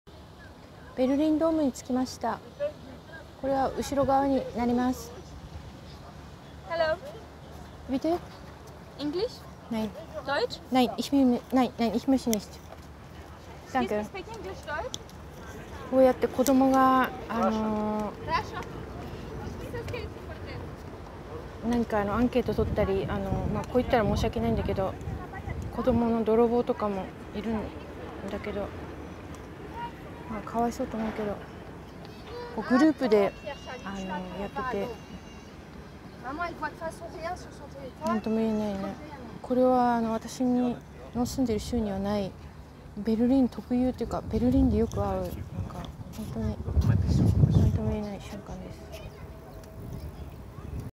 こどもがこどもとして生きられない社会の闇～ベルリンの事情
ベルリンドーム前でこどもに声をかけられました。

こどもが泥棒や詐欺に犯罪に手を染めています。

大人が組織的にさせていて、前回ベルリンに来た時もこどもの集団泥棒がいました。

悲しい現実です。